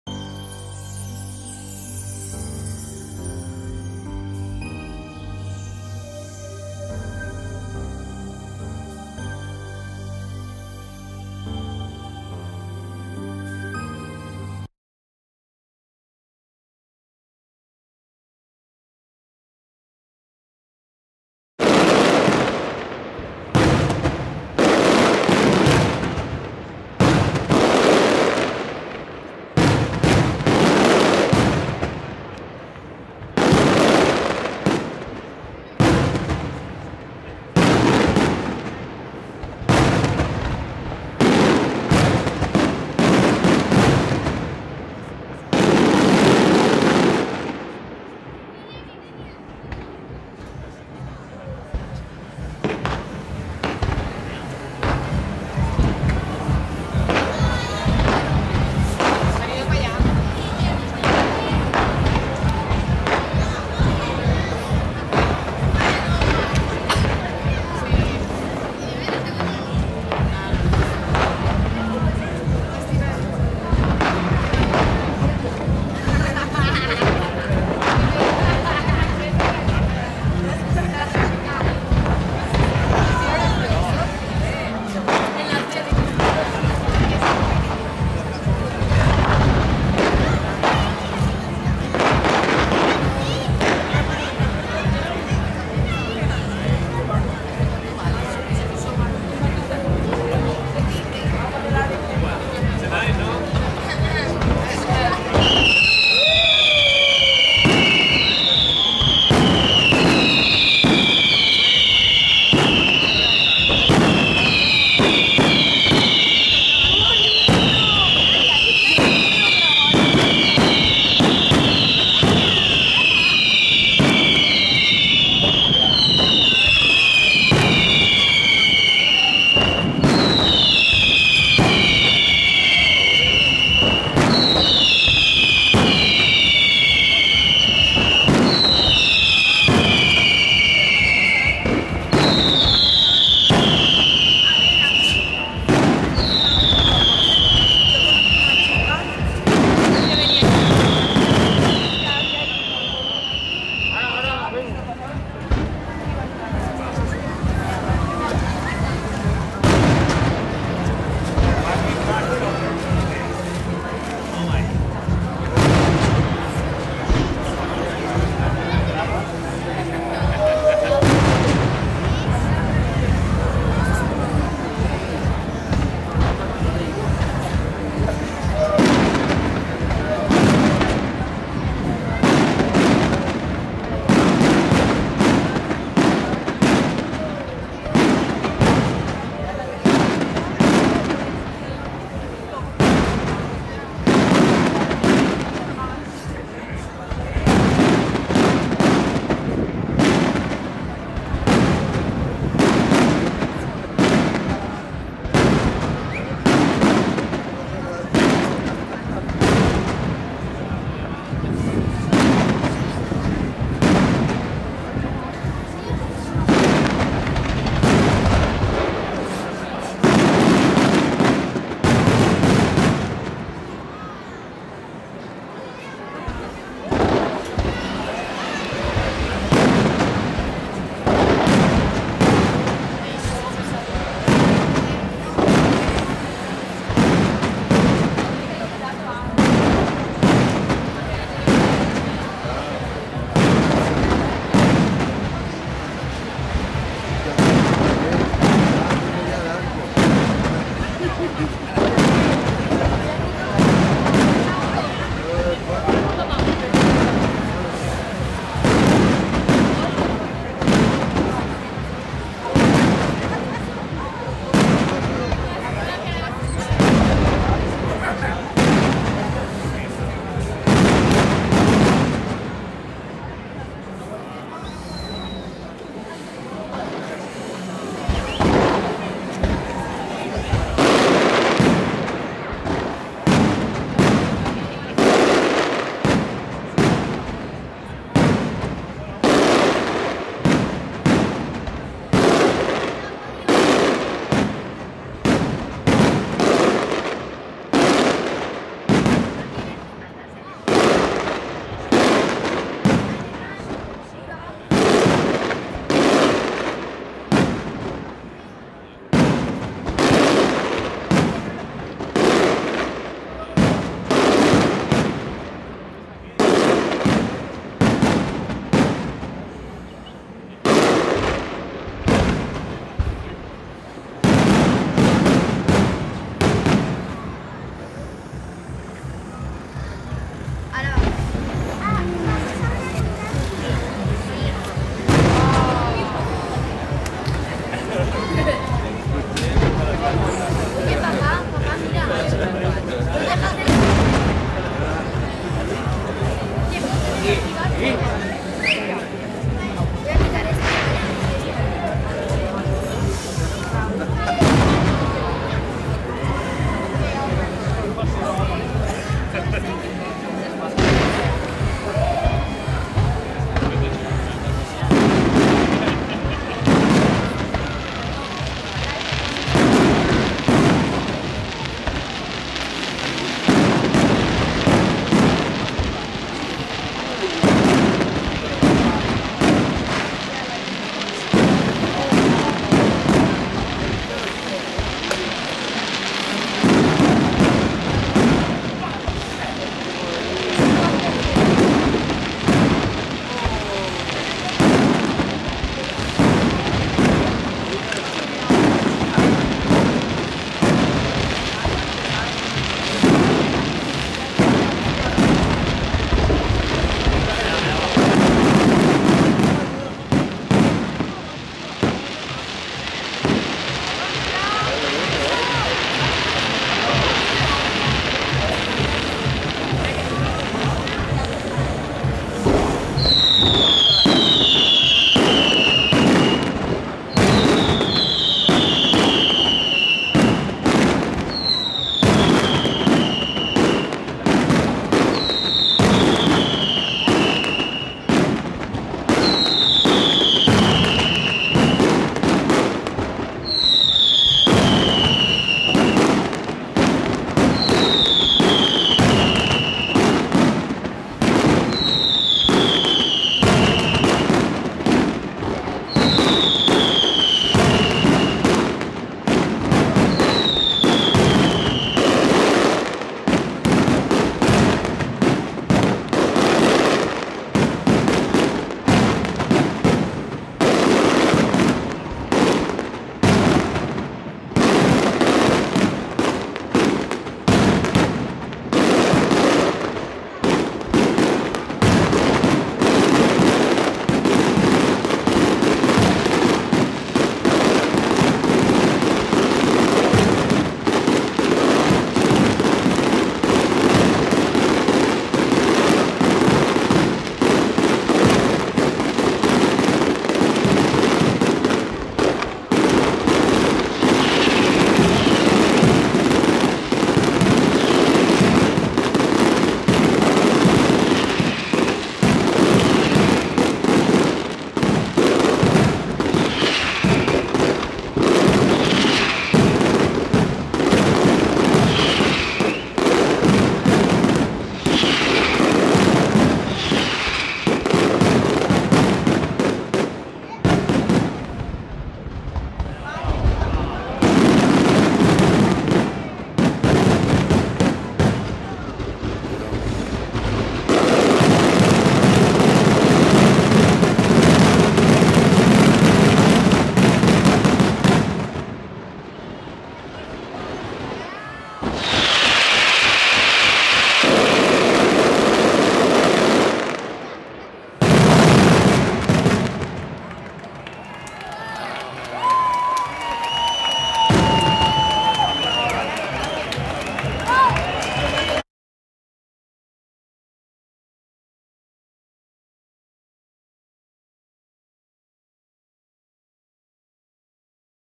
CASTILLO FUEGOS ARTIFICIALES FALLA ELS BARRIS DE TURIS 2014 P TURIS
Fragmento Castillo Fuegos Artificiales Falla Els Barris Turis 2014. Pirotecnia Turis SL